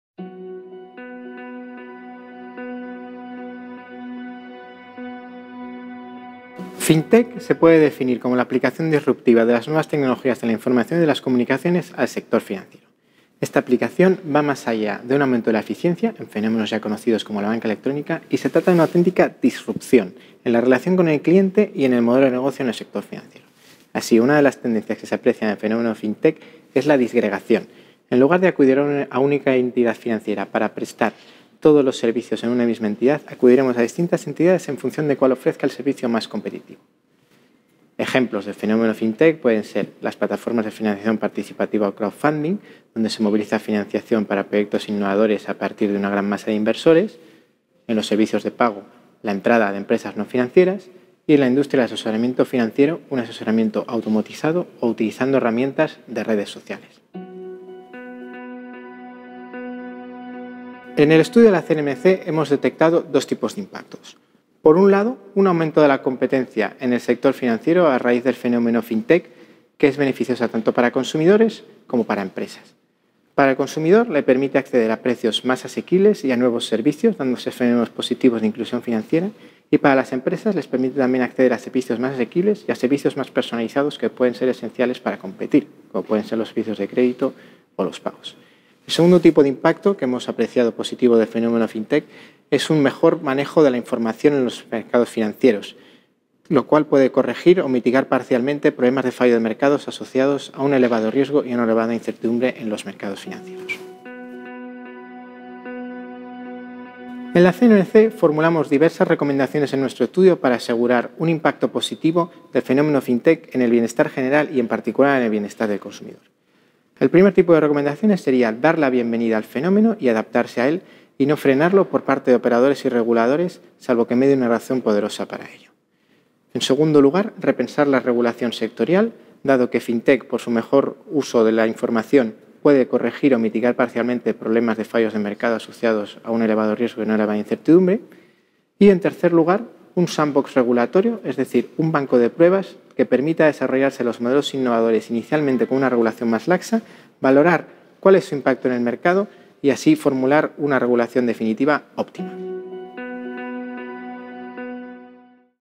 ¿Qué es FINTECH? La CNMC te lo explica.
Os presentamos el estudio sobre el impacto de las nuevas tecnologías en el sector financiero que ha elaborado la CNMC (Comisión Nacional de los Mercados y la Competencia). Contamos con las explicaciones de Pedro Hinojo, Vocal Asesor de la Subdirección de Estdios e Informes